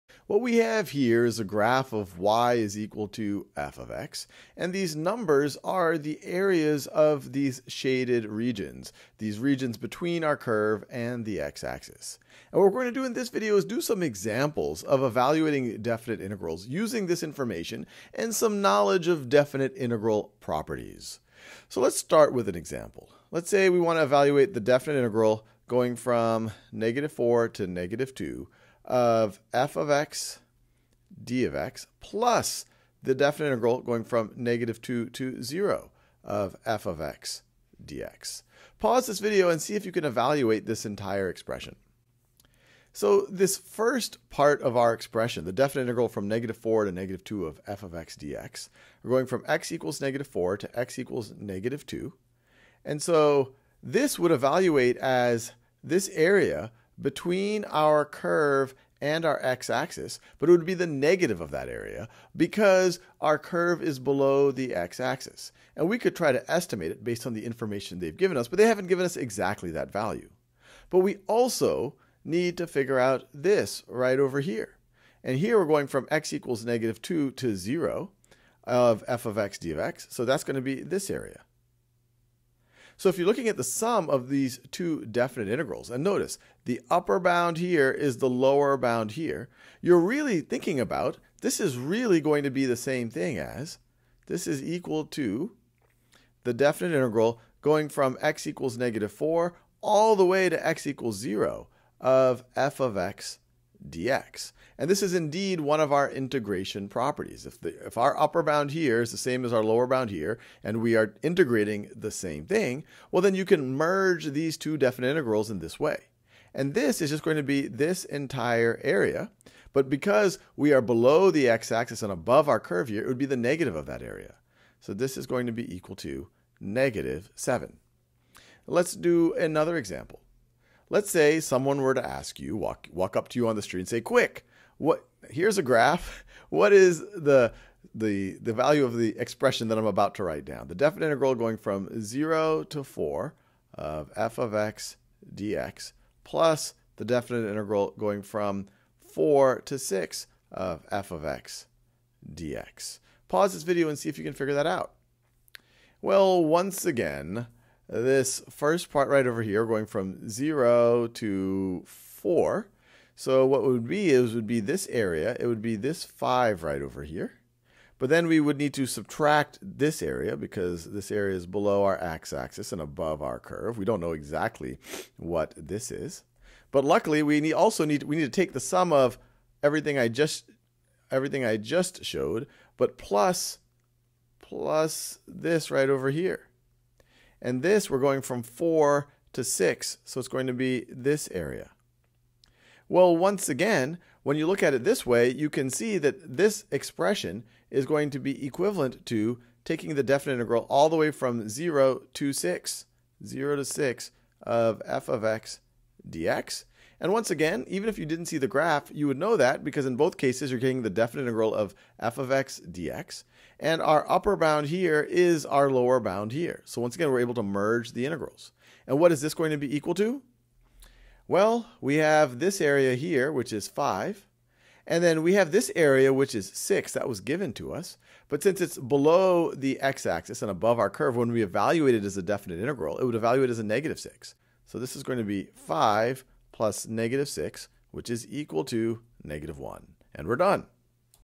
Worked example: Merging definite integrals over adjacent intervals | AP Calculus AB | Khan Academy
Courses on Khan Academy are always 100% free. Start practicing—and saving your progress—now

Thinking about how to evaluate the sum of definite integrals over adjacent intervals.